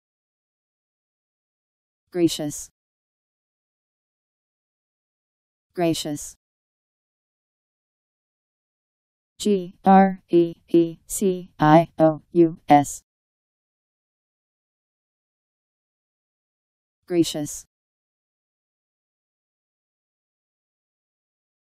Greecious Meaning
Video shows what greecious means. gracious.  Greecious Meaning. How to pronounce, definition audio dictionary. How to say greecious. Powered by MaryTTS, Wiktionary